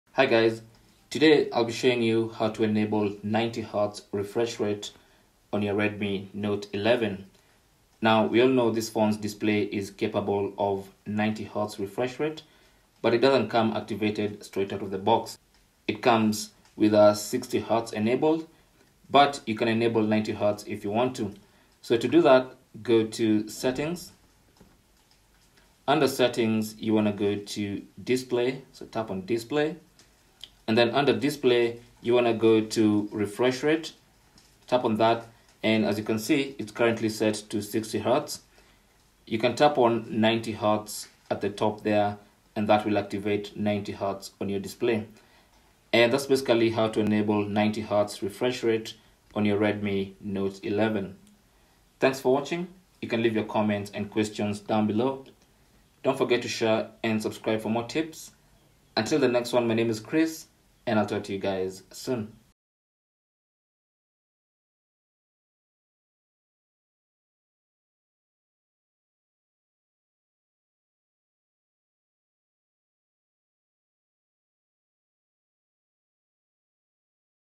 Redmi Note 11: How To Enable 90Hz Refresh Rate
The Redmi Note 11 comes with a 90Hz refresh rate display. But it's not enabled by default. Here's how to turn on that crispy 90Hz refresh rate.

90Hz will look and feel smoother especially in gaming but at a cost of faster battery drain.